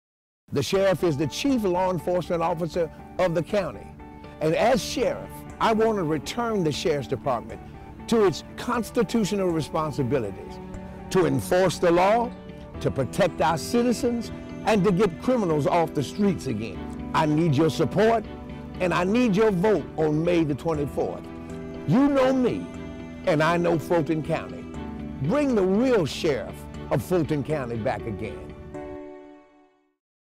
SHERIFF LANKFORD FOR FULTON COUNTY SHERIFF
Bring the real Sheriff back! Vote in the May 24th primary!